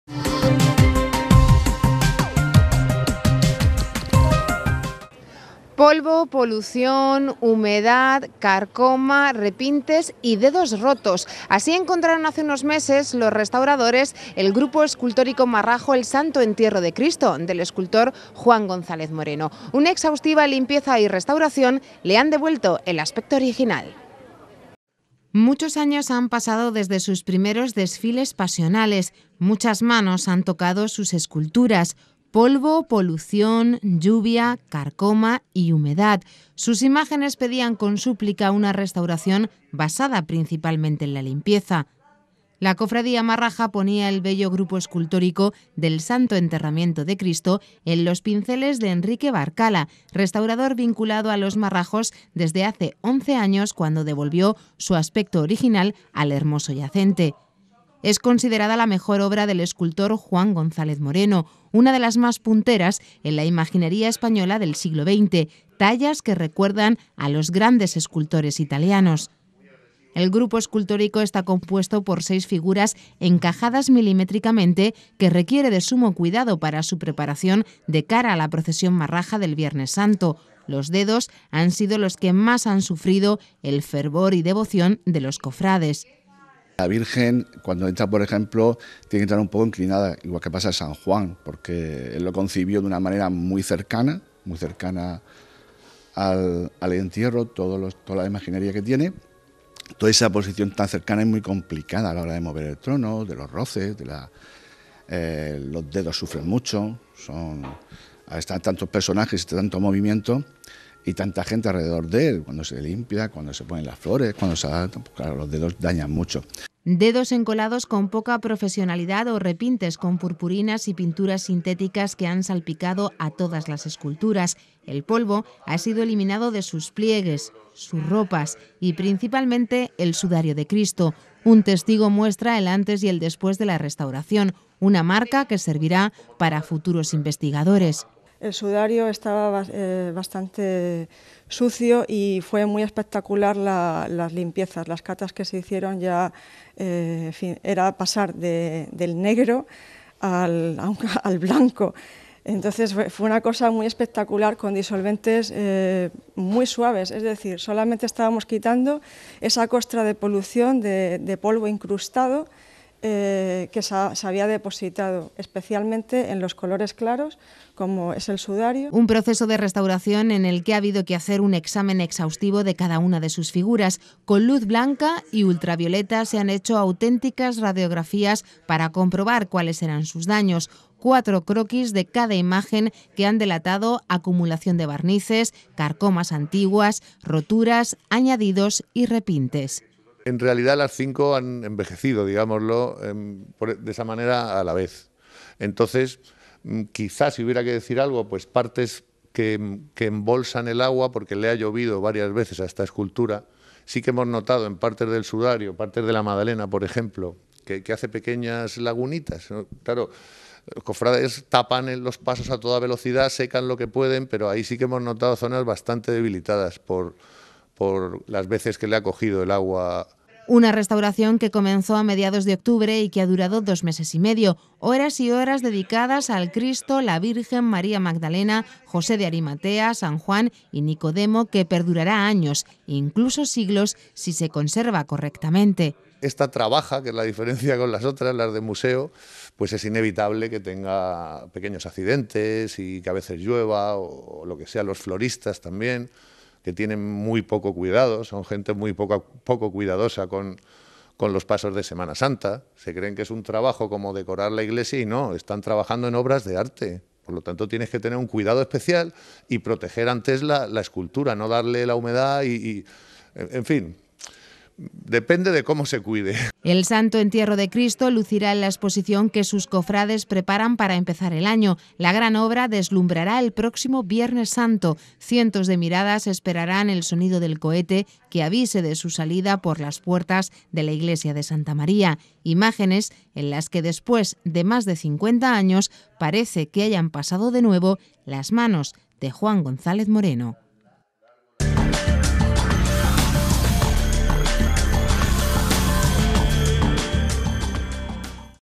Restauración Santo Entierro de Cristo
Los restauradores devuelven el aspecto original al grupo escultórico marrajo el Santo Entierro de Cristo de Juan González Moreno